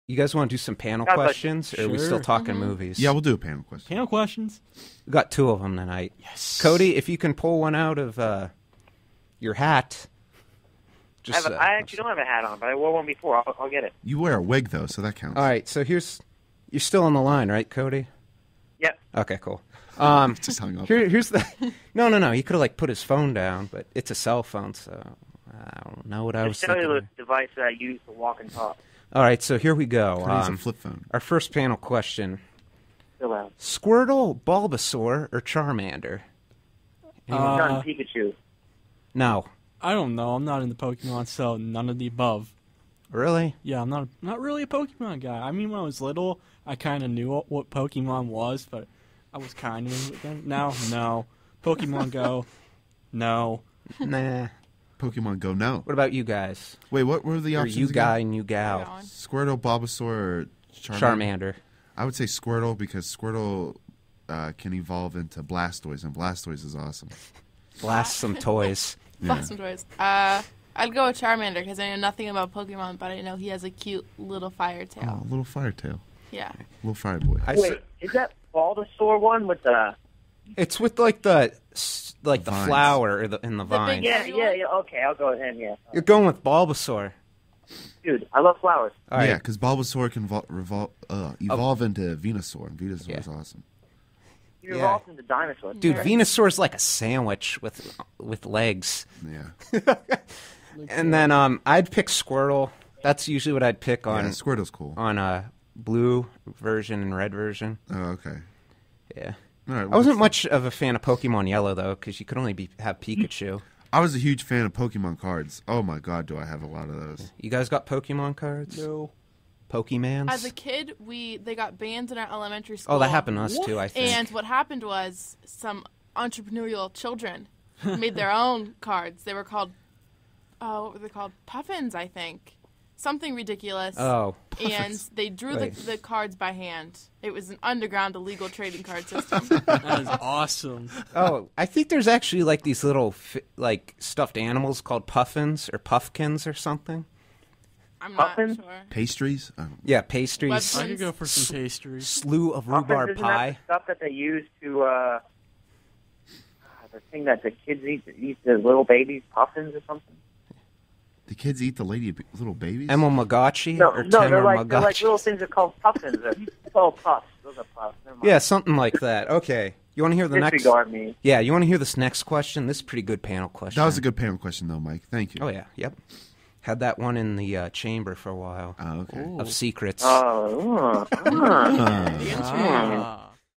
Worry Talk, 09/22/16 - Squirtle, Bulbasaur or Charmander?
Mike asks everyone (Scott, Brendan and Cody) which Pokemon they would pick from the original (1st gen) Pokemon Game Boy games and why they would pick them.